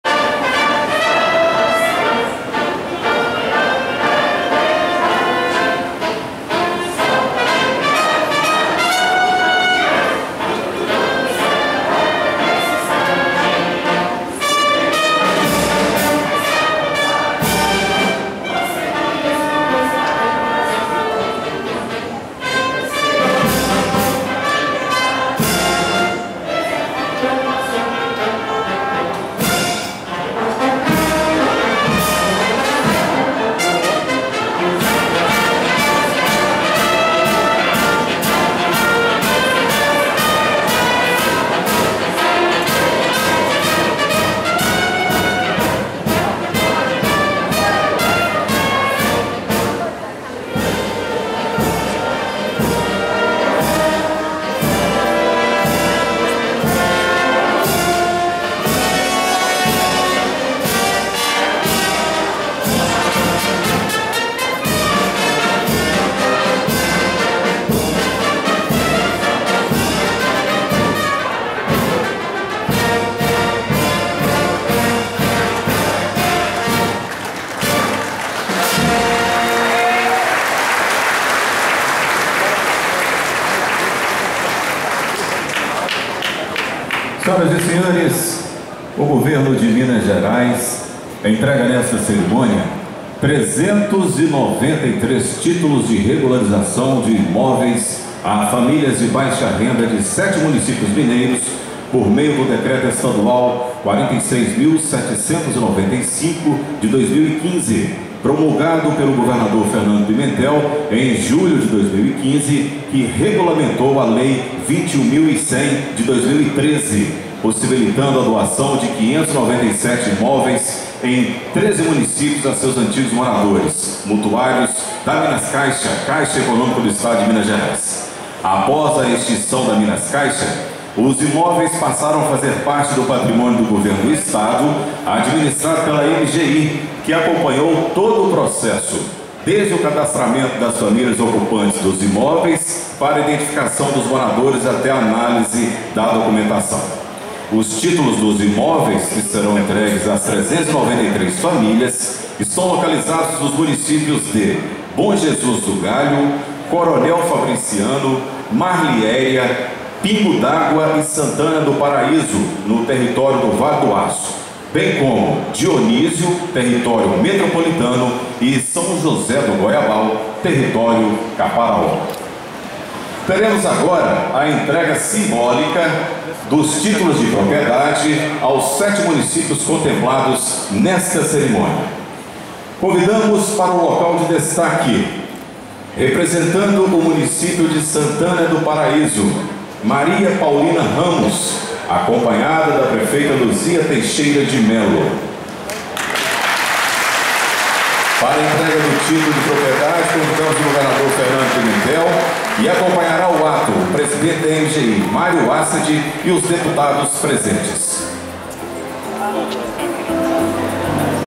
Solenidade com Governador de Minas - Mutuários da Antiga Minas Caixa
pingodaguamg Governador de Minas - Pimentel faz onde entrega títulos de propriedade de 393 imóveis a famílias de baixa renda de sete municípios daquela região. Os beneficiados são ex-mutuários da extinta MinasCaixa.
O benefício visa a legalização de imóveis e é uma luta de mais de uma década dos moradores do bairro conhecido hoje como Residencial Paraíso, que contou com o esforço de três ex-prefeitos do município de Santana do Paraísoao ao longo desse tempo.

Além de Santana do Paraíso serão beneficiados pela medida, mutuários residentes em Bom Jesus do Galho, Coronel Fabriciano, Dionísio, Marliéria,  Pingo D'Água e São José do Goiabal. A regulamentação da doação dos imóveis de que trata a legislação estadual foi feita por decreto do governo do estado.
A Lei que autoriza o estado a executar a doação dos imóveis foi aprovada em 2013 na gestão do então governador Antônio Anastasia.